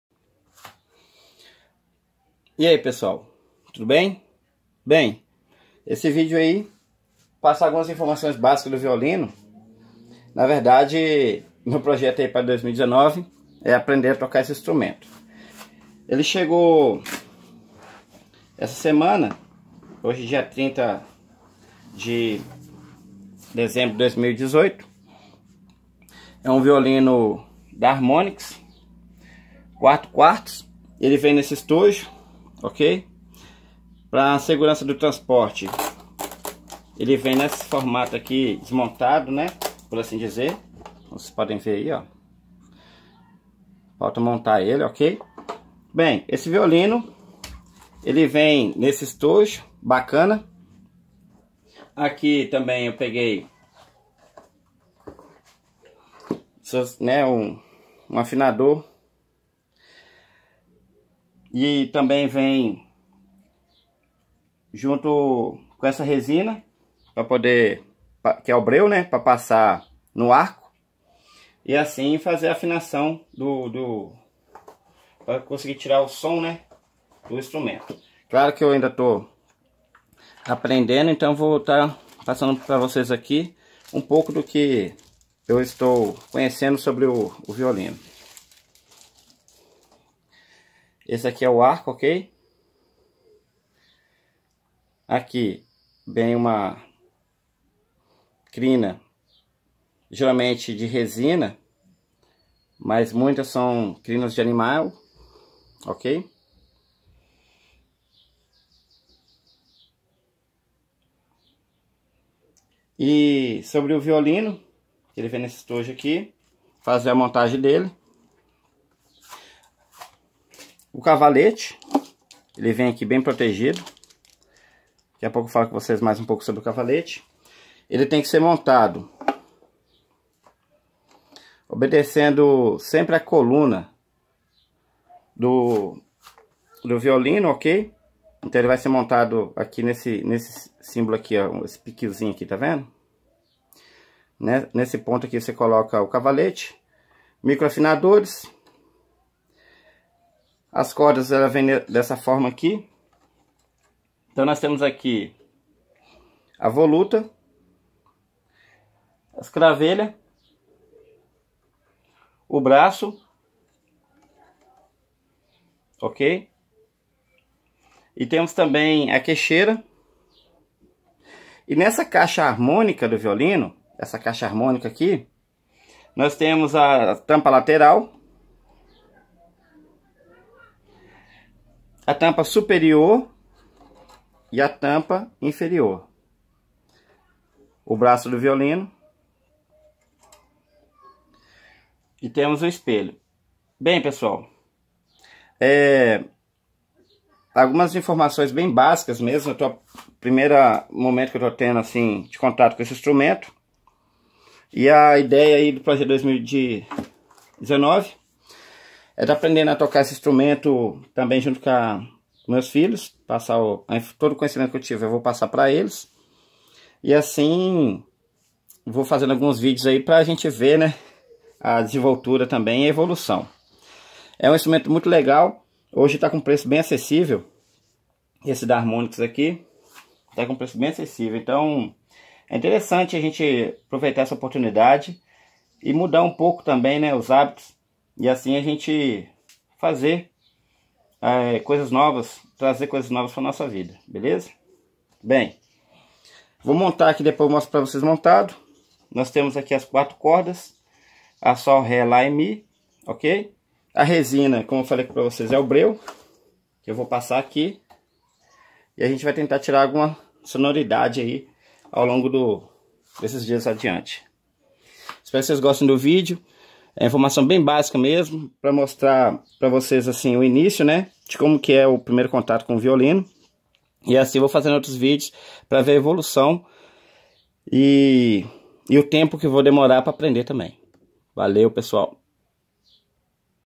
Violino Clássico Harmonics VA-10 4/4, Primeiro Contato.
Informações básicas e primárias deste instrumento que apresenta boa relação custo / benefício para os iniciantes.
Além da beleza nos detalhes do acabamento, apresenta boa qualidade em todo seu conjunto.